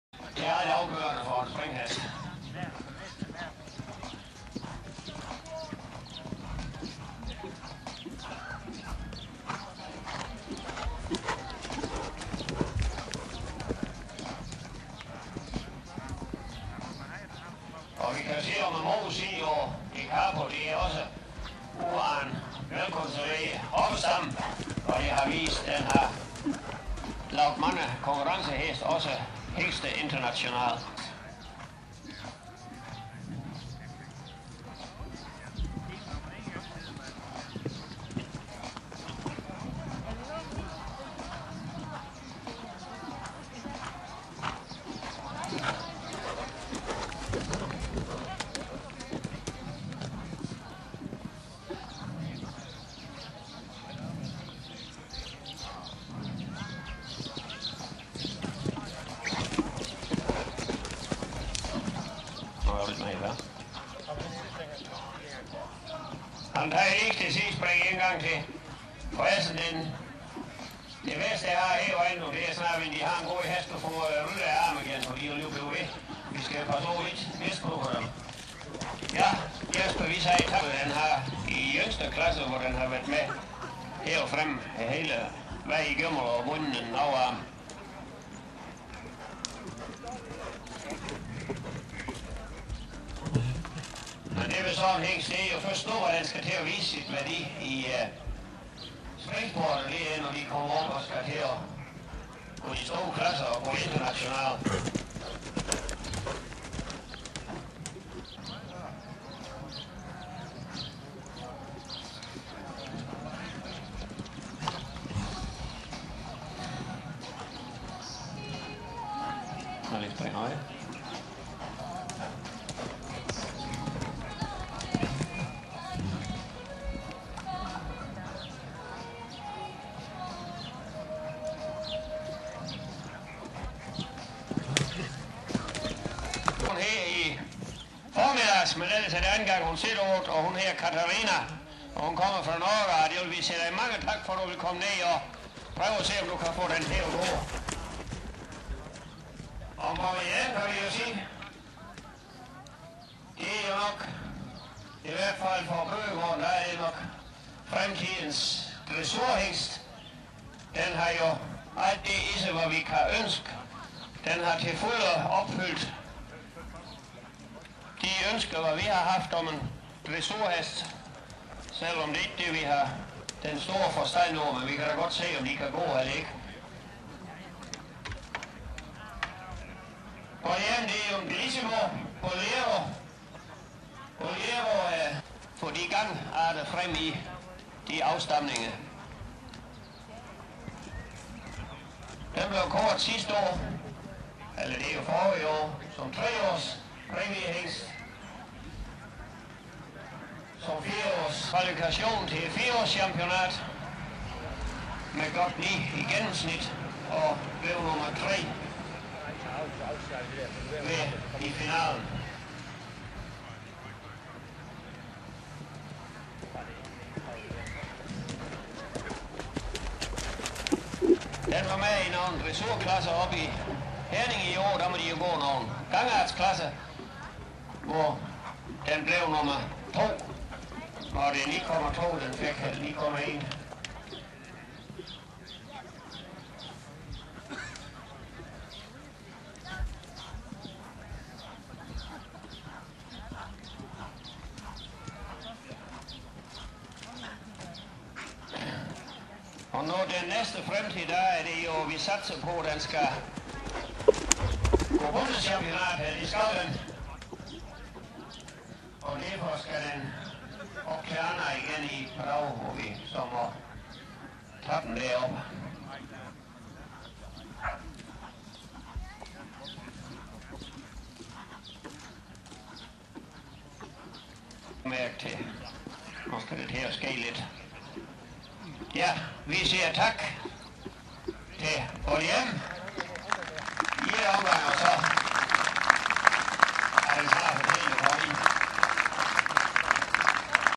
Åbent Hus på Bøgegården 2012
Se hingstene Dicapo, Graf's Stakkato og Boney M.